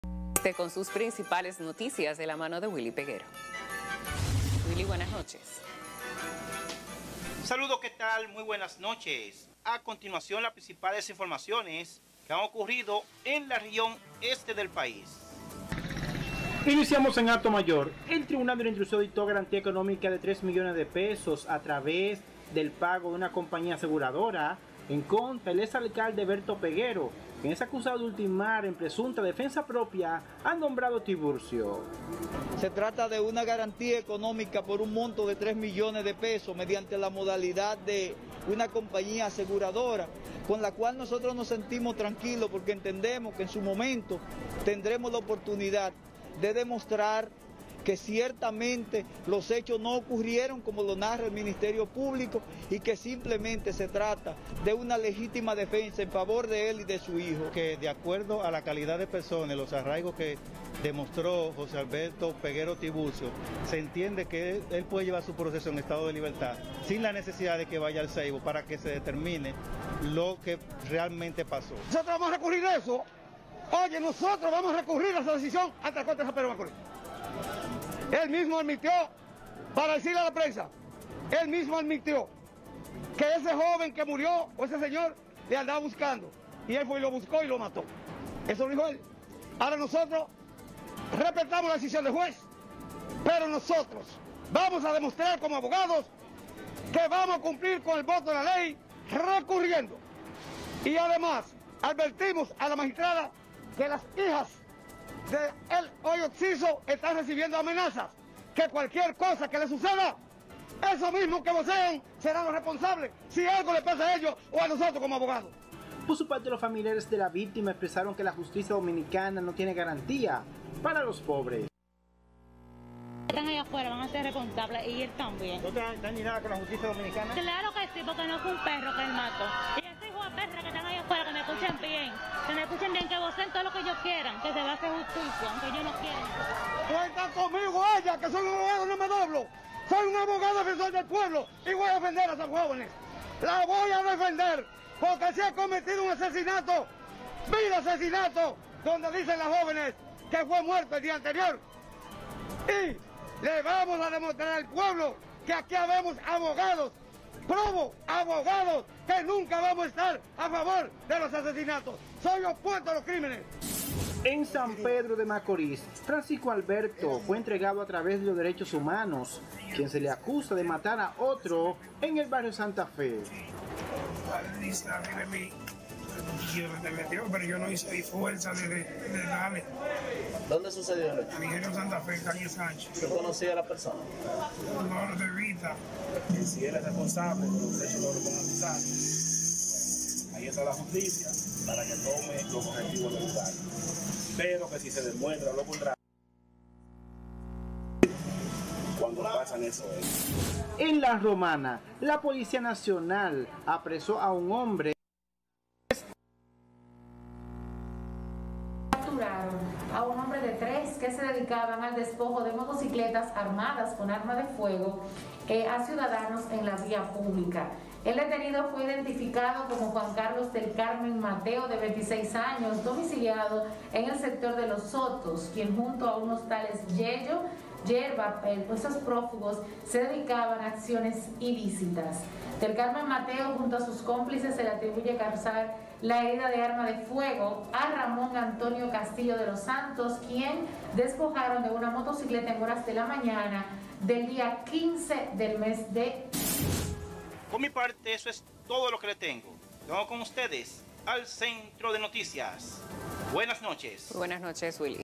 ¡Las noticias más relevantes de la región Este!
A continuación los detalles en el siguiente informe.

Es momento de conocer las noticias más relevantes de la región Este con el periodista Willy Peguero.